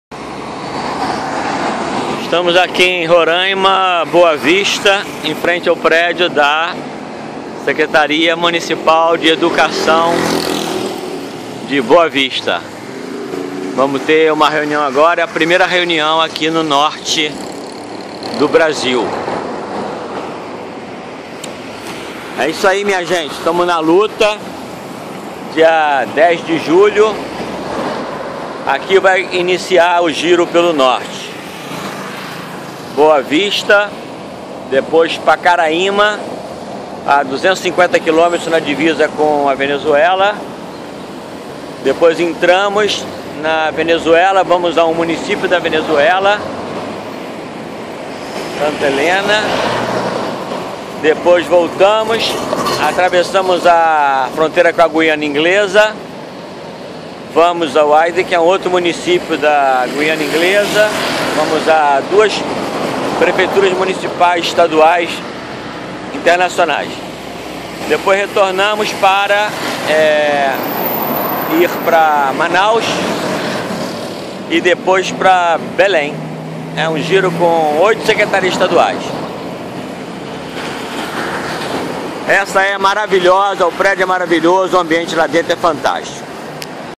IPAI PELO NORTE EM 2014
Mauricio Menezes, Presidente do IPAI em julho de 2014 em um giro pelo Norte do Brasil para levar as ESCOLAS DE CIDADANIA para o Amazonas, Pará com Ilha de Marajó, Roraima e Venezuela e Guiana Inglesa. Pacaraima o município mais ao Norte do país recebeu a sua.